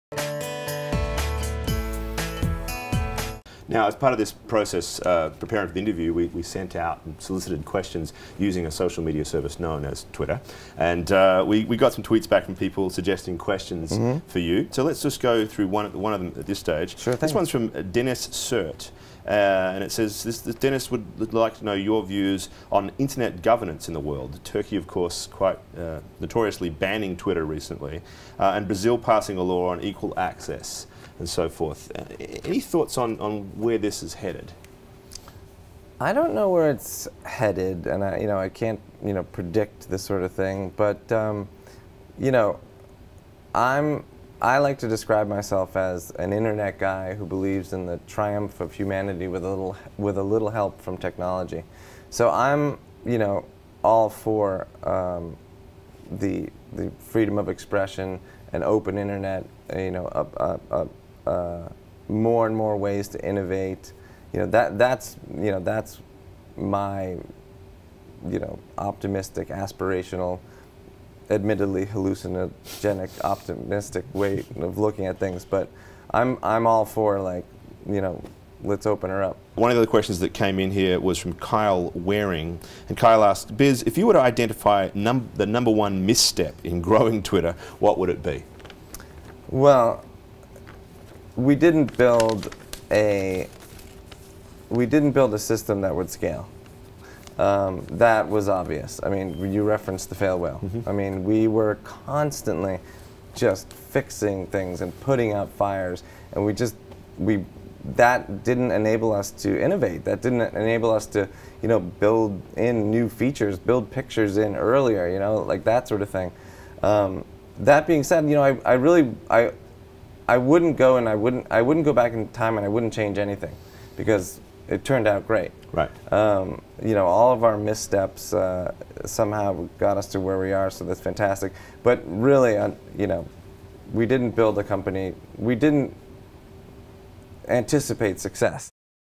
Stone Responds To WSJ Twitter Questions
Twitter co-founder @Biz Stone responds to questions from WSJ Twitter followers on Internet censorship and the missteps in Twitter's development.